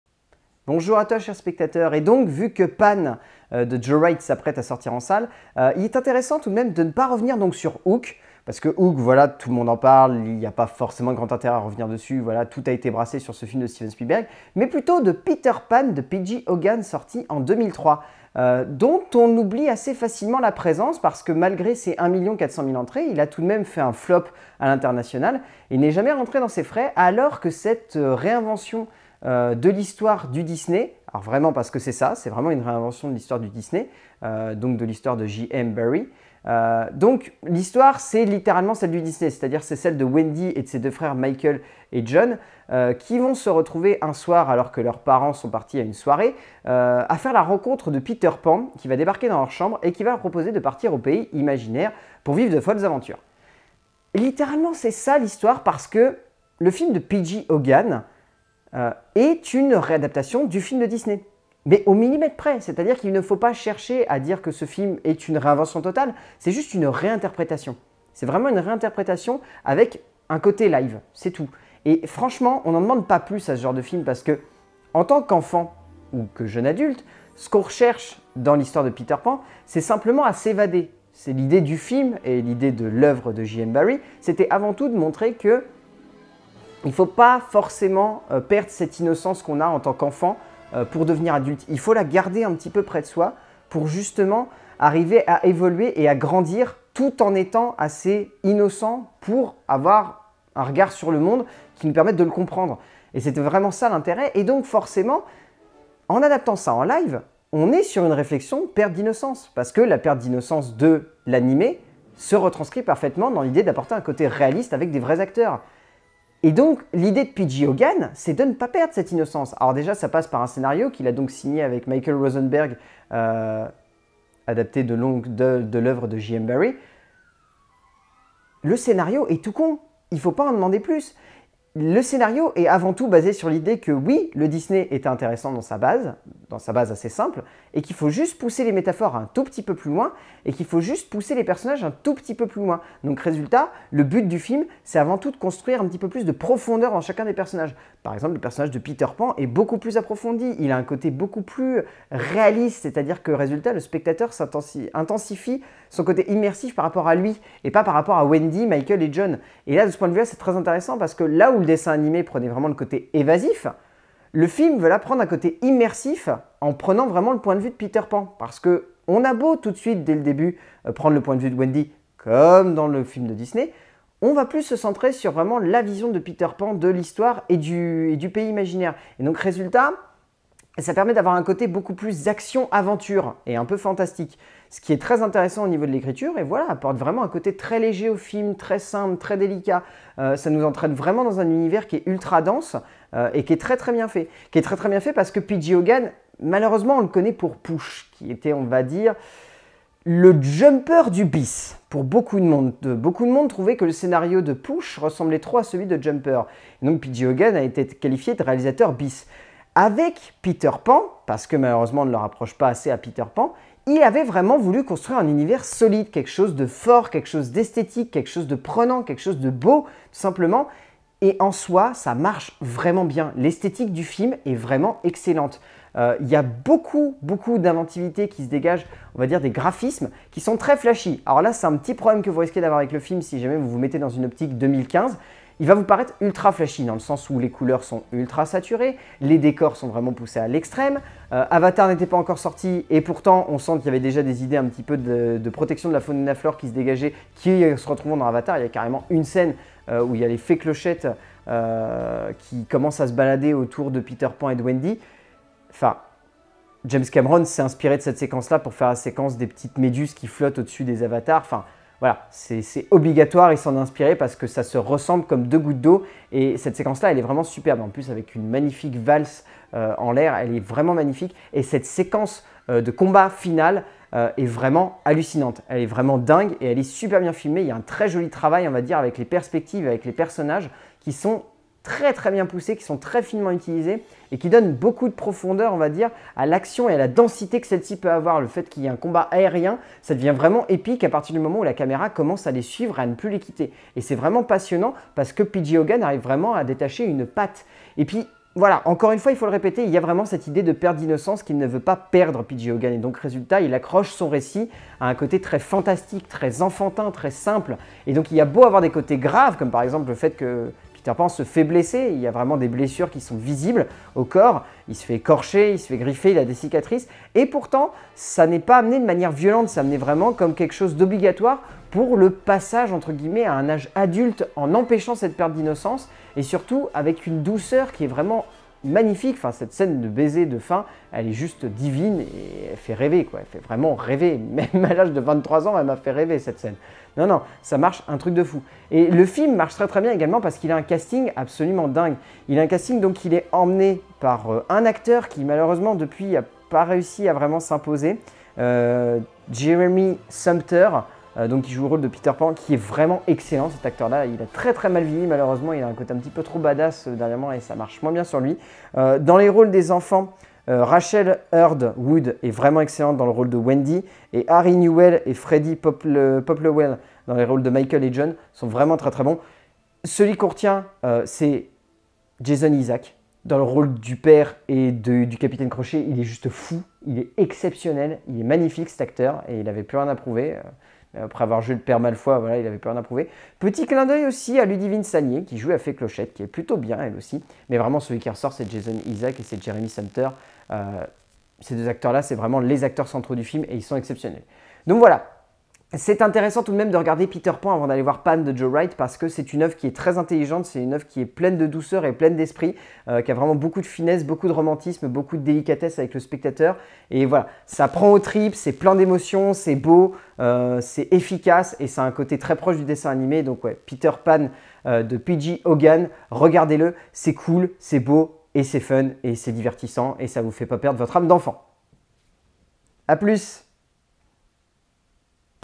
LCDC - Peter Pan (2003)
Vlog critique revenant sur la sortie il y a maintenant plus de 12 ans, d'un film qui est passé un peu inaperçu en france, et qui pourtant, rendait terriblement bien hommage à un classique du cinéma d'animation, "Peter Pan"